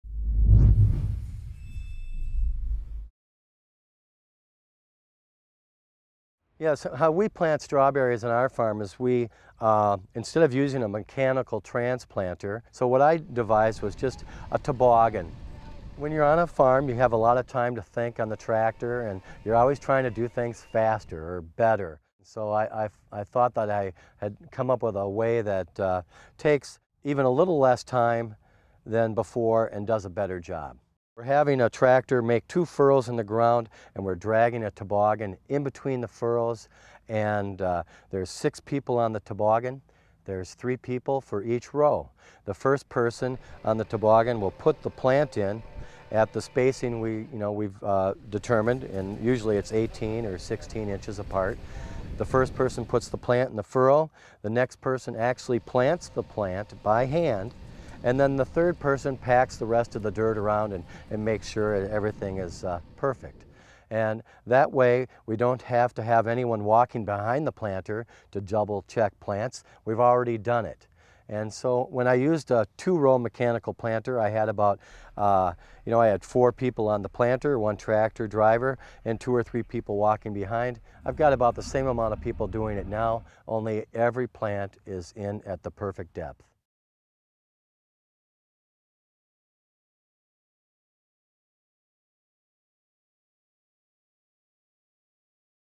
A Toboggan Planter for Planting Strawberries
University of Minnesota farmer-cooperator Bill Johnson shows his homemade toboggan planter, which makes planting strawberries faster and easier on his farm.

Part of the University of Minnesota Cold Climate Strawberry Farming video series.